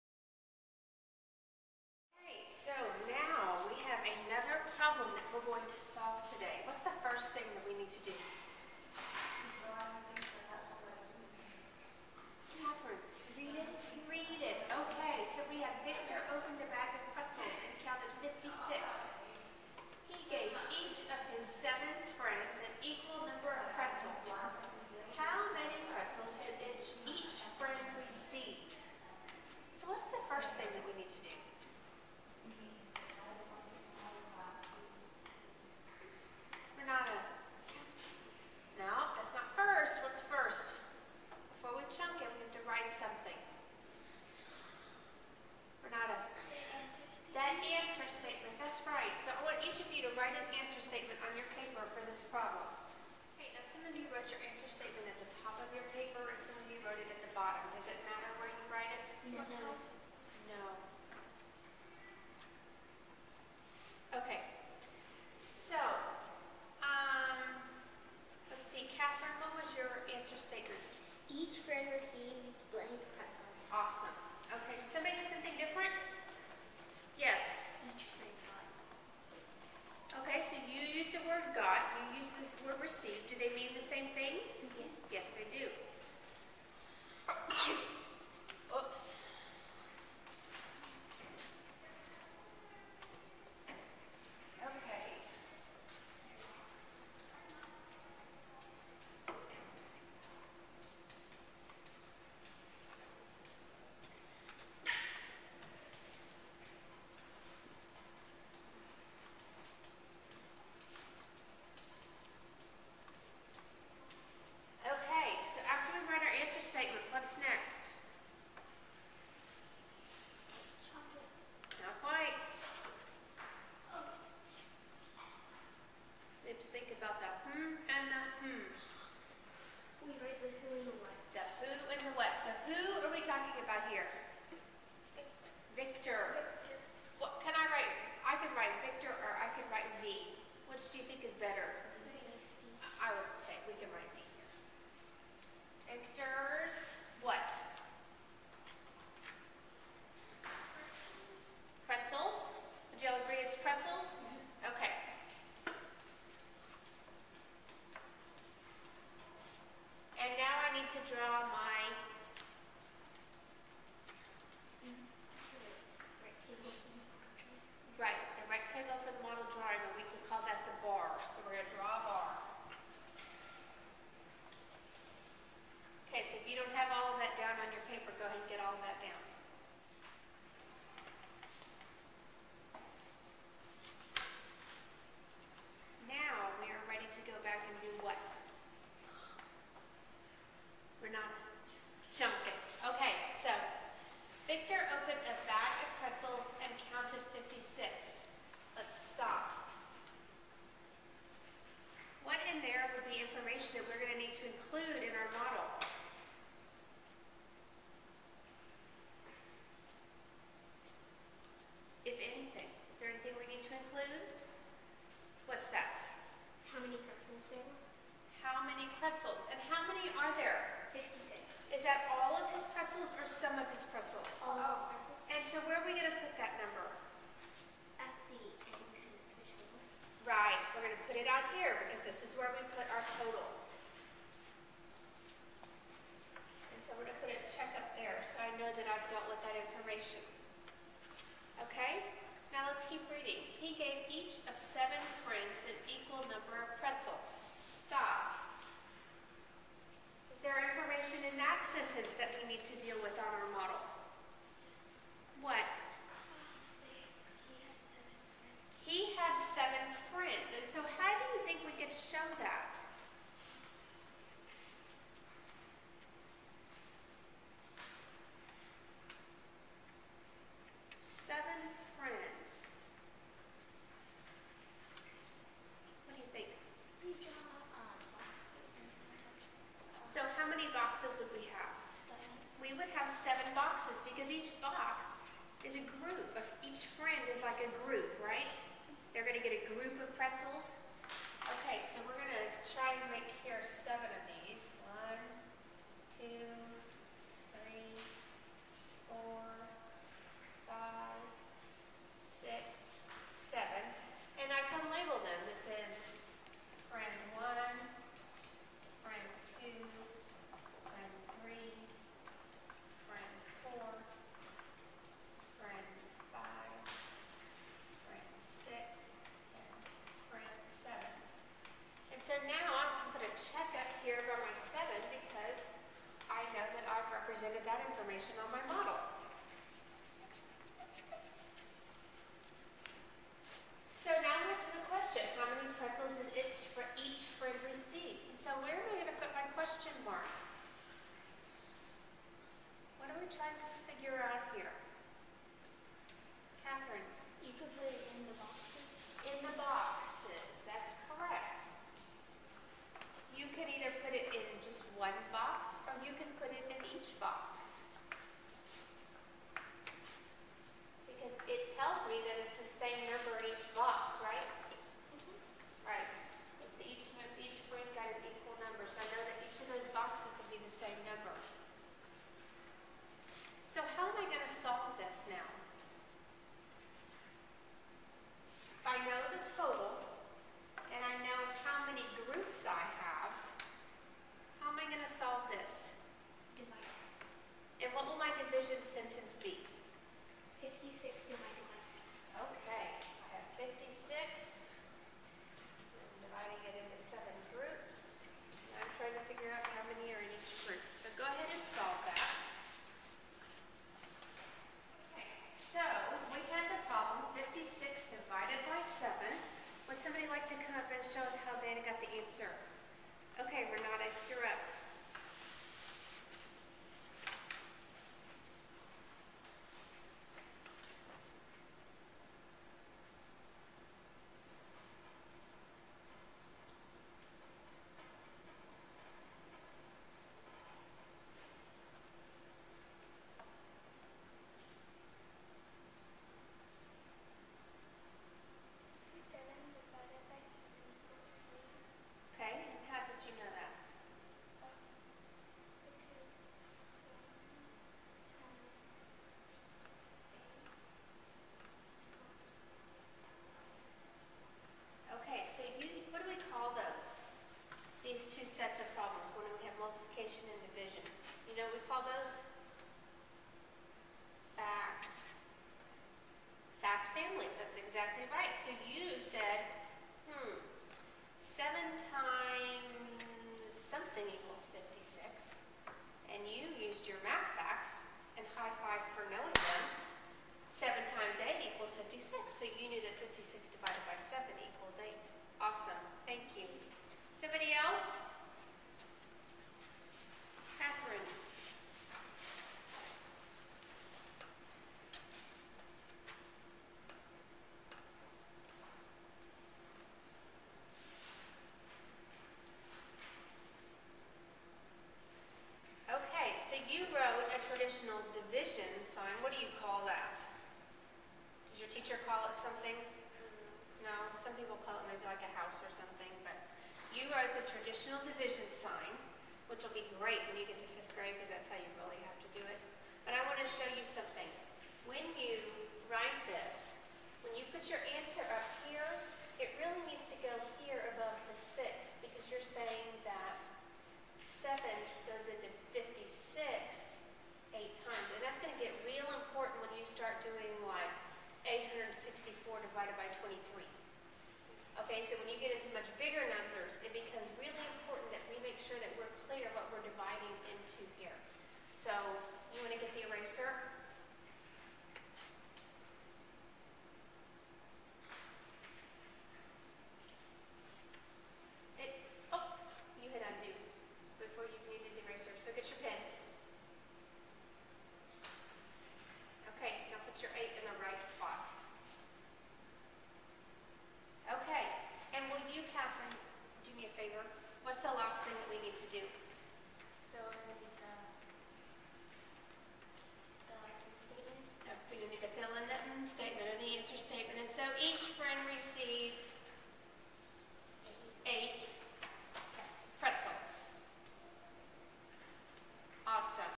Division Findng the Number in Each Group
TeacherTube User: Mathrockskallen

Use Singapore Math Model Drawing for division when you are finding the number in each group.